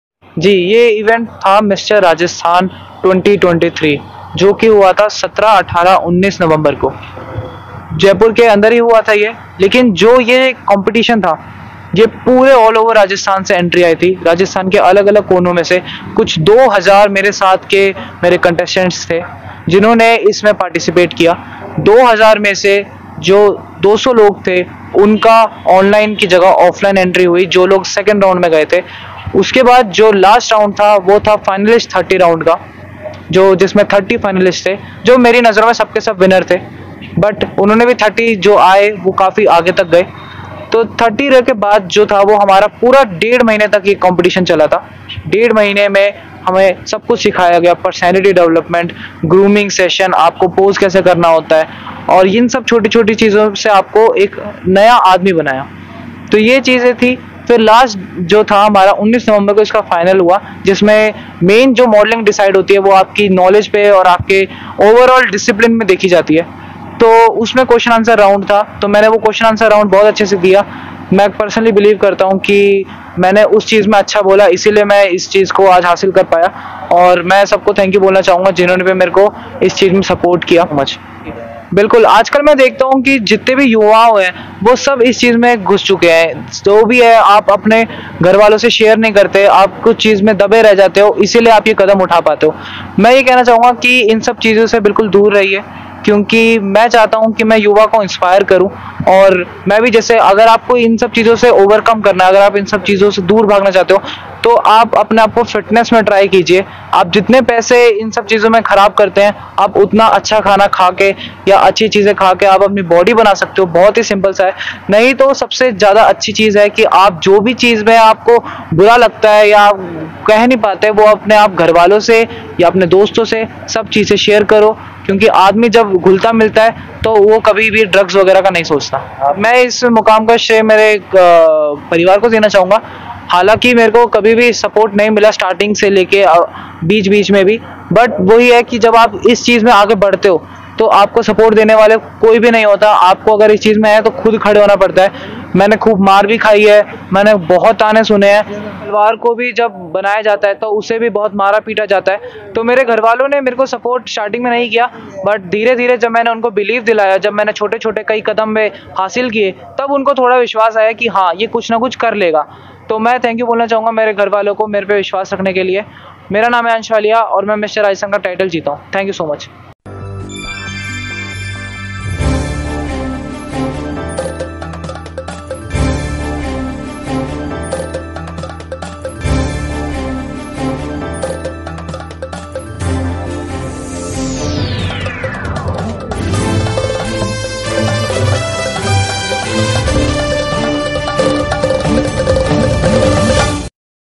घर वालों ने युवक को ताने दिए पर युवक ने हौसला नहीं छोड़ा मिस्टर राजस्थान प्रतियोगिता जीती ,सुने
घर वालों ने युवक को ताने दिए पर युवक ने हौसला नहीं छोड़ा मिस्टर राजस्थान प्रतियोगिता जीती ,
बीते वर्ष नवम्बर में हुई मिस्टर 2023 राजस्थान प्रतियोगिता के विनर रहे अंश वालिया को साथ लेकर एक निजी होटल में प्रेस वार्ता का आयोजन किया गया। मिस्टर राहस्थान रहे अंश वालिया प्रतियोगिता जीतने के बाद पहली बार ग्रह जिला हनुमानगढ़ में आये। वहीं अंश वालिया का जगह-जगह स्वागत भी किया गया। पत्रकार वार्ता के दौरान विनर रहे अंश वालिया ने युवाओं को नशे से दुर रहने का भी सन्देश दिया। प्रेस वार्ता के दौरान अंश वालिया के पिता उमंग वालिया,डॉक्टर सन्तोष राजपुरोहित, सामाजिक कार्यकर्ता सावन पाइवाल, अधिवक्ता नितिन छाबड़ा और नवनीत शर्मा उपस्थित हैं।